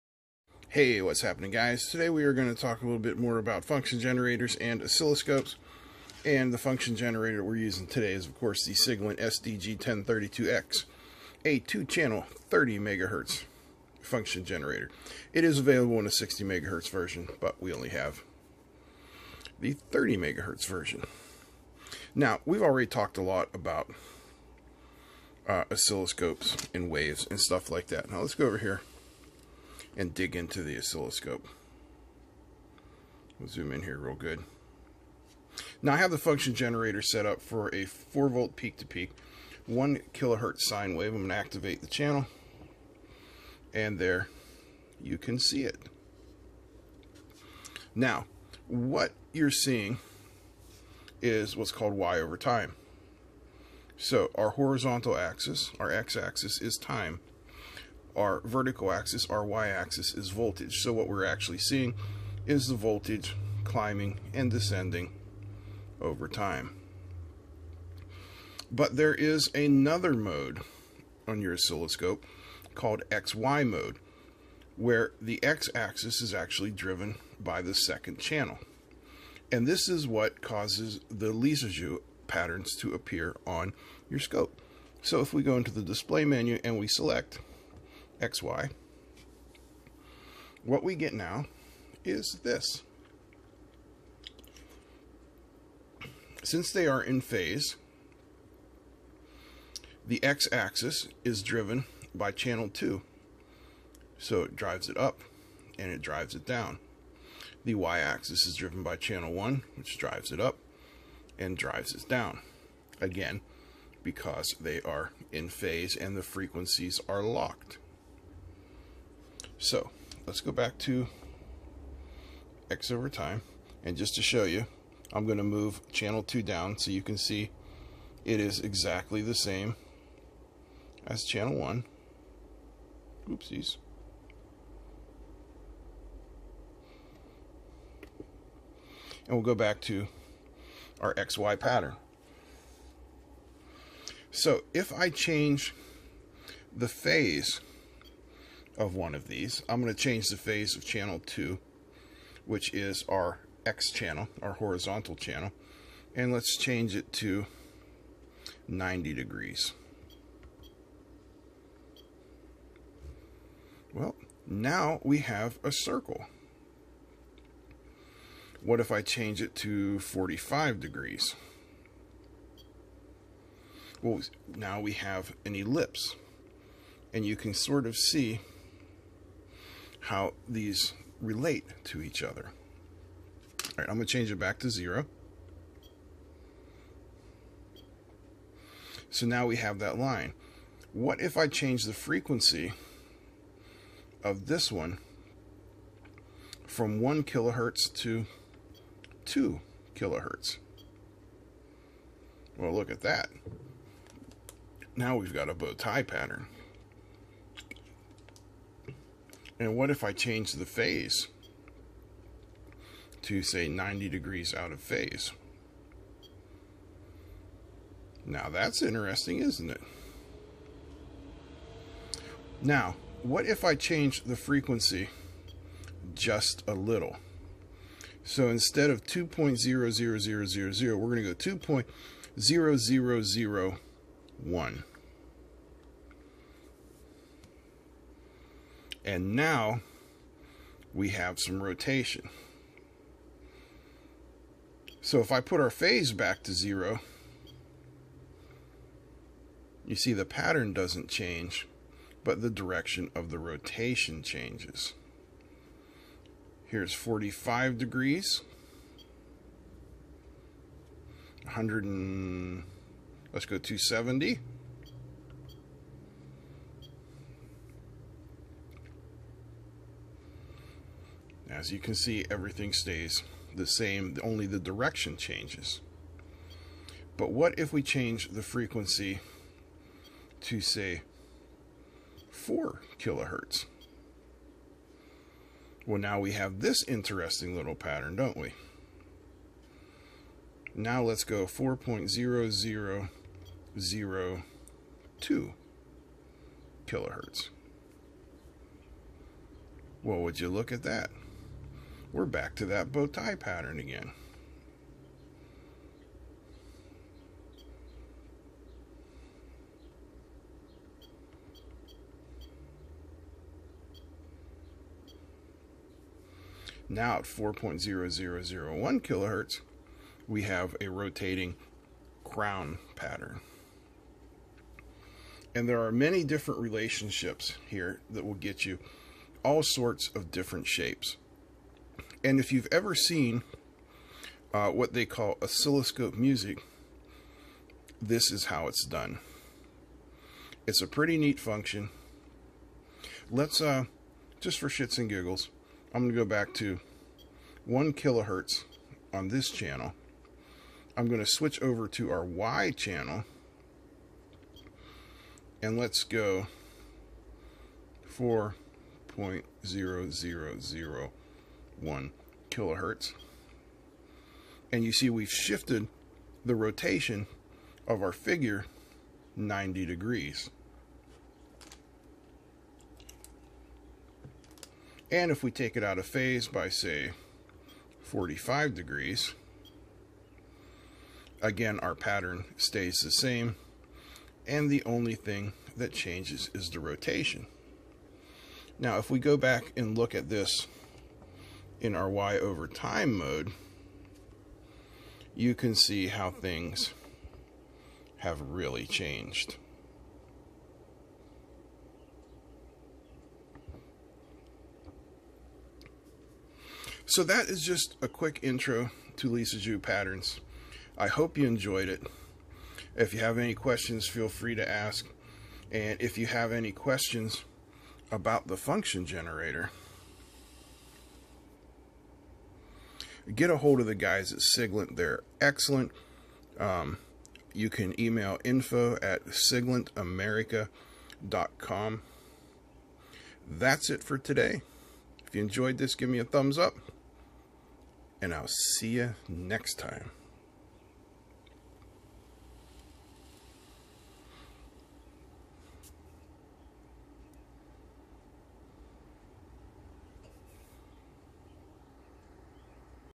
What are Lissajous patterns
What is a Lissjous Pattern on a 'scope?

A Lissajous pattern  is the graph of a system of parametric equations which describe complex harmonic motion. In electronics, we can view these on an oscilloscope by switching from our standard viewing of voltage/time (Y/T) to X/Y mode.
In this video we use the Siglent SDG1032X dual channel function generator to create sine waves for each channel, then by changing the frequency, phase, or both of one of the channels we can produce the patterns.
If you have ever seen "oscilloscope music" this is how it's done.